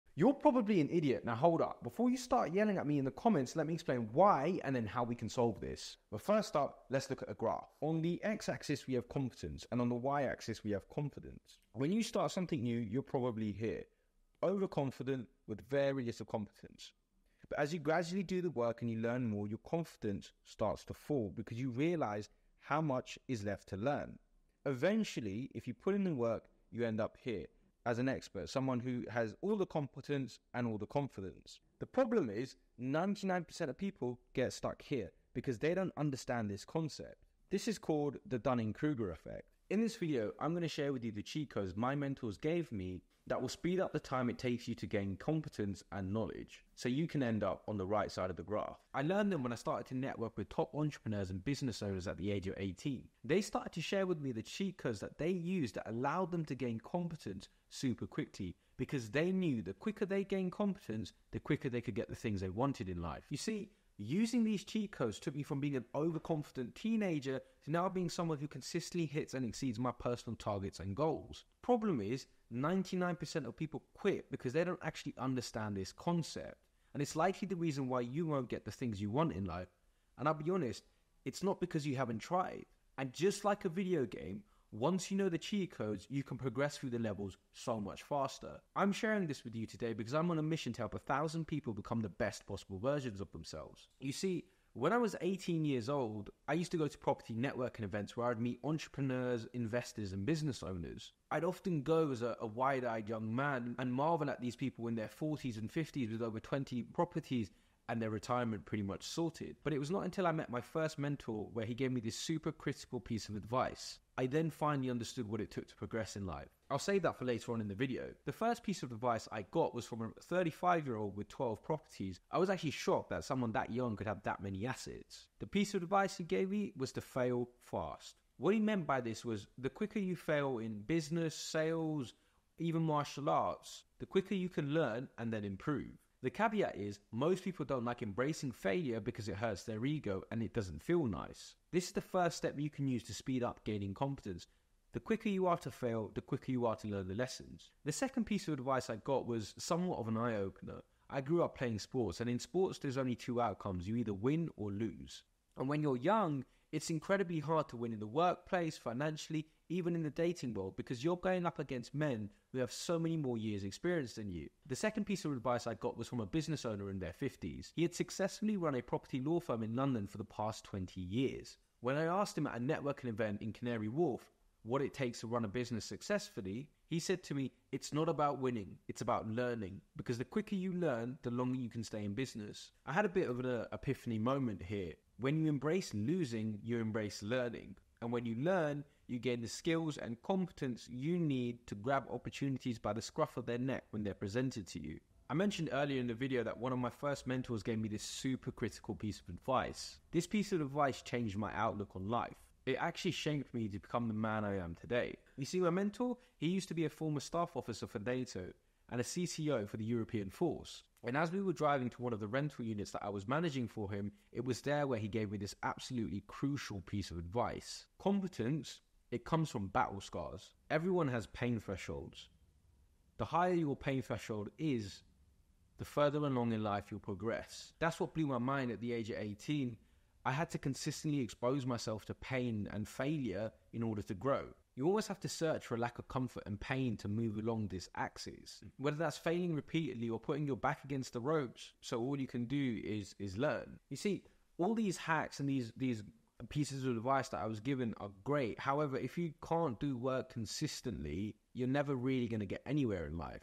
Why Stupid People Think They're Smart
This is called the Dunning-Kruger Effect.

Chapters:
What is the Dunning Kruger Effect: 0:00 - 0:40
CheatCode 1: 2:00 - 2:27
CheatCode 2: 2:28- 3:15
CheatCode 3: 3:15- 4:17